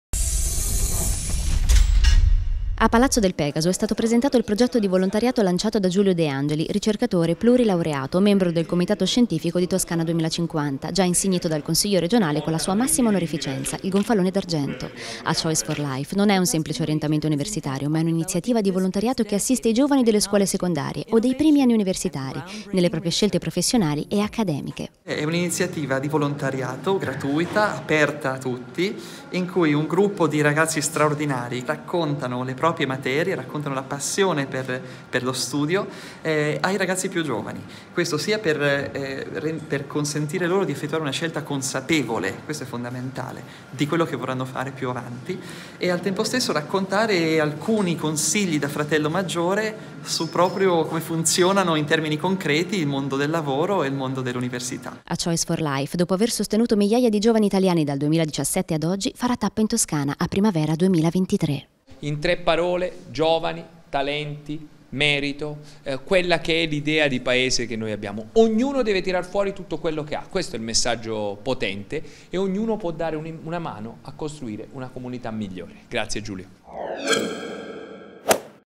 A choice for life 2023: l'orientamento universitario innovativo ideato dal neuroscienziato Deangeli
A Palazzo del Pegaso è stato presentato il progetto di volontariato lanciato da Giulio Deangeli, ricercatore plurilaureato, membro del comitato scientifico di Toscana 2050, già insignito dal Consiglio regionale con la sua massima onorificenza, il Gonfalone d’argento.

‘A choice for life’ non è un semplice orientamento universitario, ma è una iniziativa di volontariato che assiste i giovani delle scuole secondarie, o dei primi anni universitari, nelle proprie scelte professionali e accademiche.

'A choice for Life' dopo aver sostenuto migliaia di giovani italiani dal 2017 ad oggi farà tappa in Toscana a primavera 2023.

Nel servizio le interviste al presidente del Consiglio regionale della Toscana Antonio Mazzeo e al neuroscienziato ricercatore della Cambridge University Giulio Deangeli.